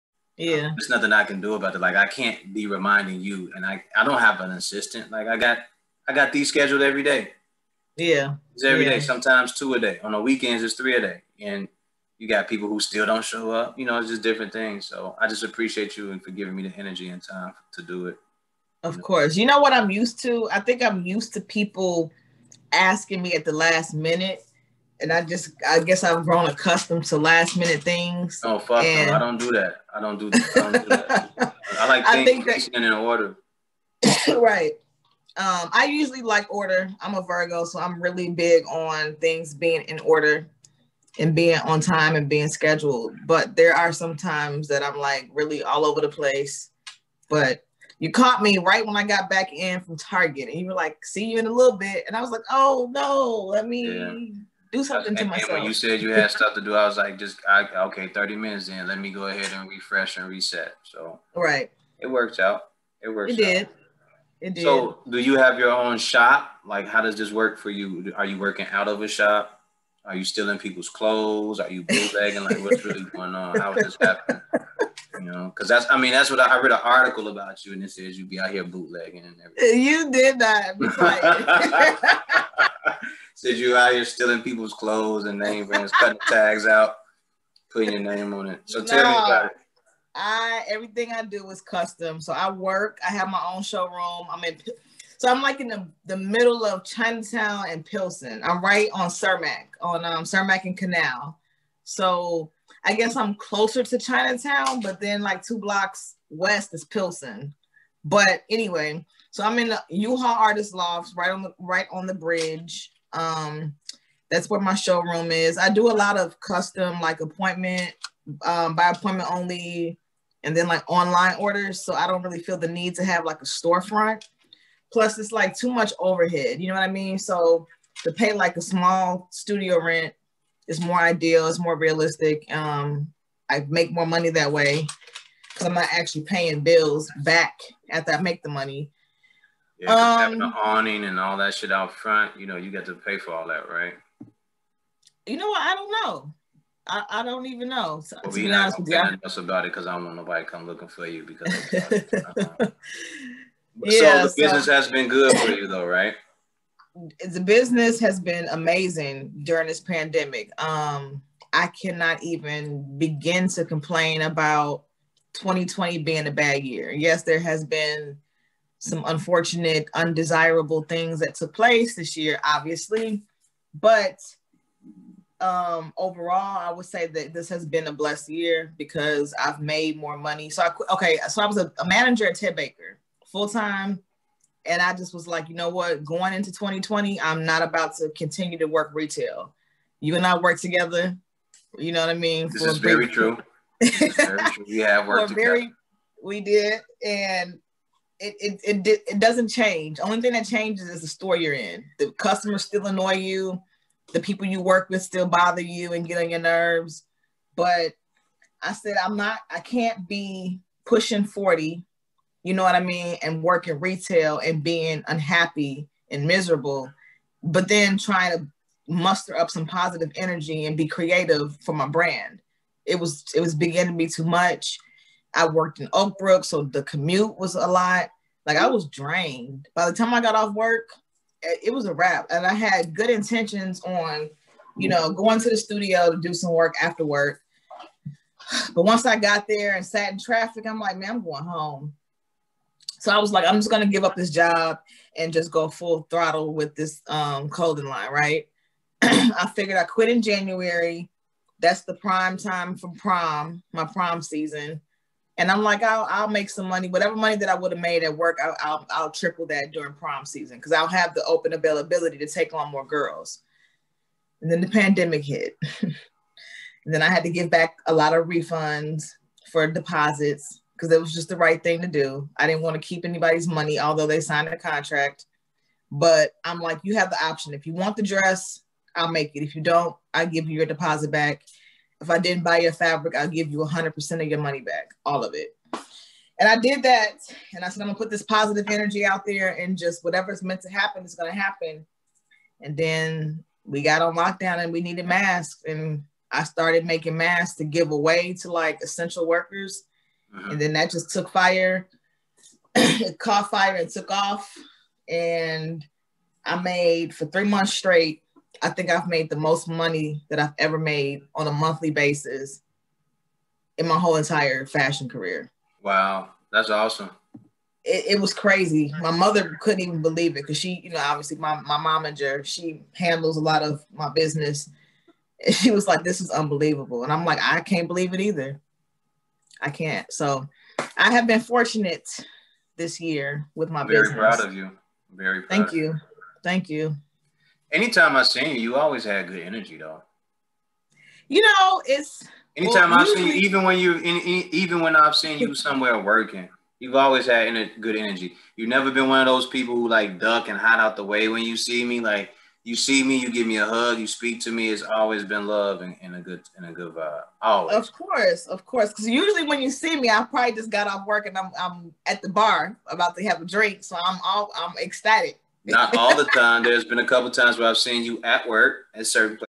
Veronica Kelly pt.1
Hey Hey! Meet Veronica Kelly! An awesome person, designer/tailor. Tune in as we talk about her independence through this COVID 19; putting the proper energy into the universe. Then standing on what you say and doing the work it takes to keep going! Positive energy has an awesome value!
And...NO! SHE DOES NOT SCAM! HOWEVER, SHE IS DOPE AT WHAT SHE DOES.
For more on the podcast, check out:
Google, iTunes, Spotify and Instagram.
Be Great!